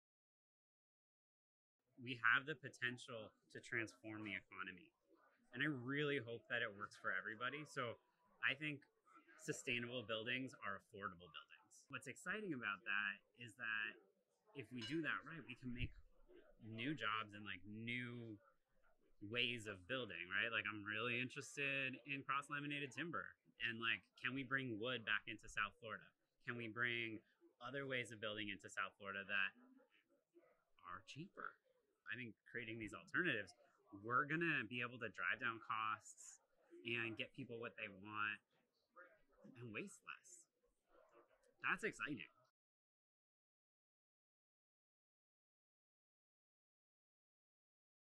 Galen Treuer Southeast Florida Climate Leadership Summit 2022 Clip
Galen Treuer, works in Climate Tech and Economic Innovation with Miami-Dade County and is excited about the future of sustainable development both for affordability and innovative materials in South Florida.